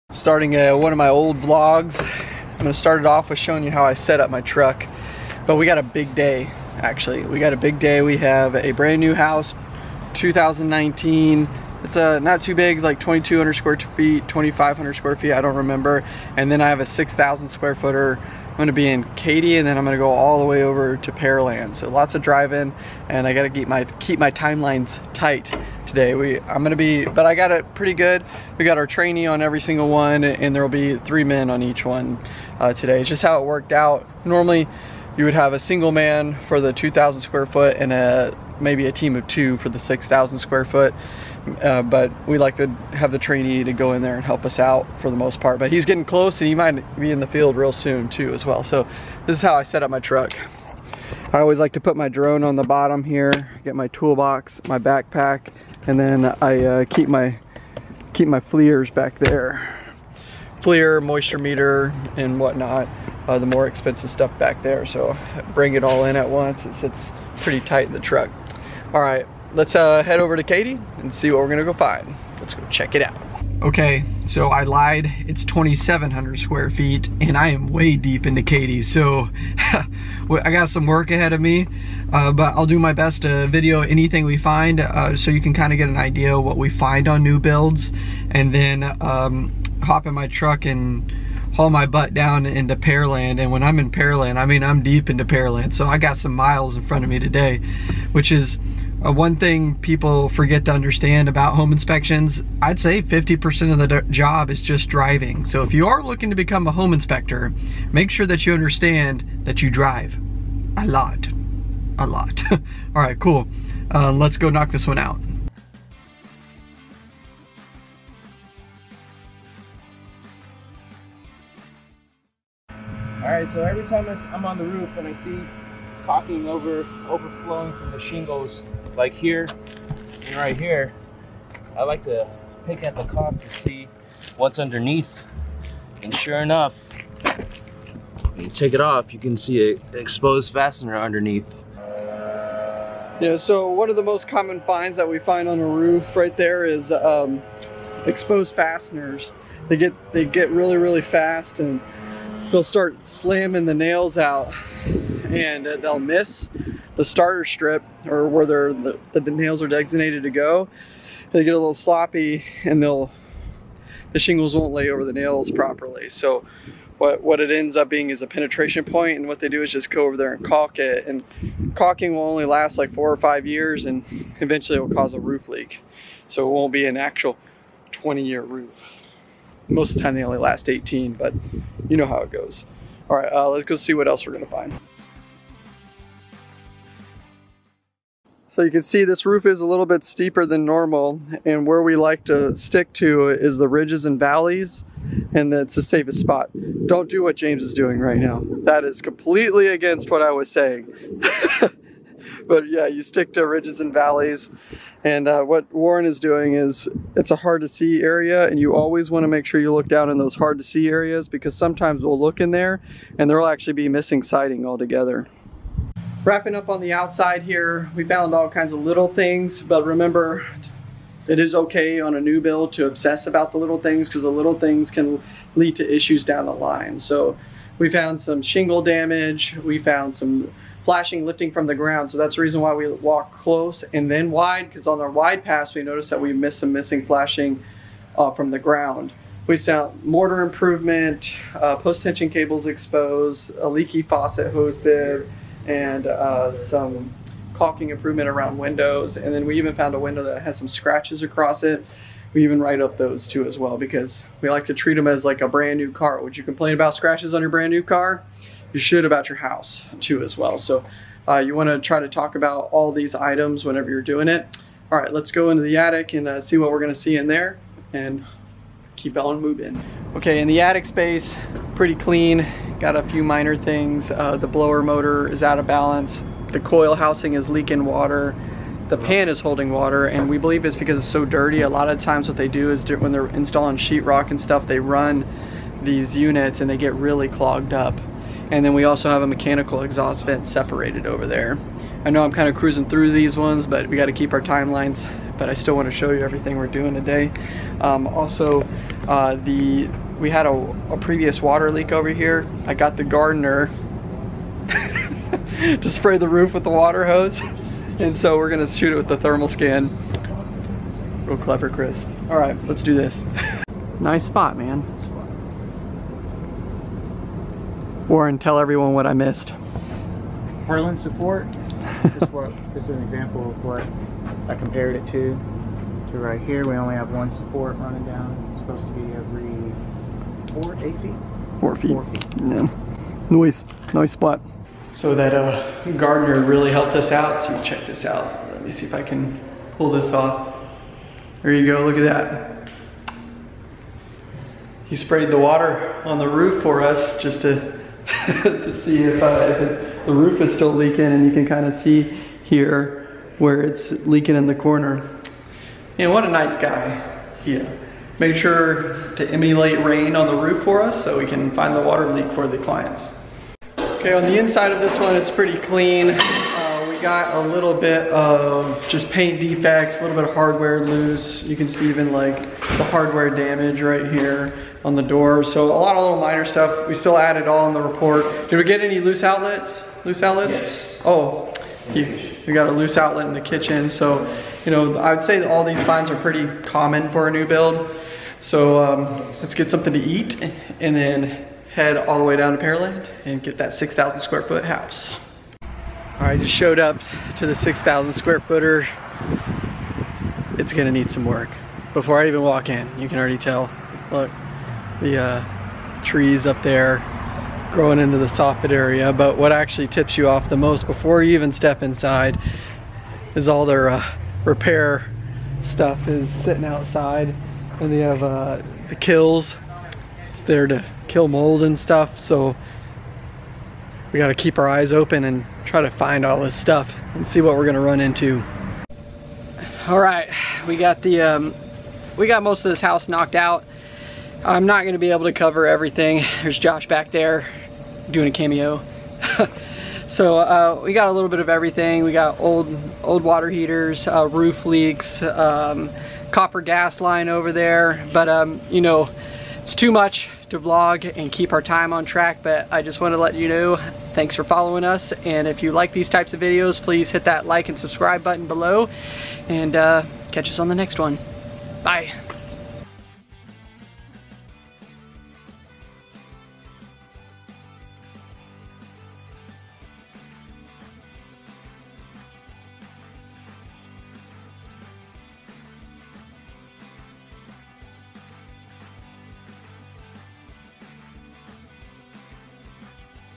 My Day As A Home Inspector - The Houston Home Inspector
In this week's video, follow me around as I go through some home inspection finds with some of my inspectors. Get a sneak peek of a day in my life! We found issues in the roof, attic, and more. Let's go check it out!

A-ActionHouston.com
HoustonTexasHomeInspection.com

Next Youtube video outcome and check it out.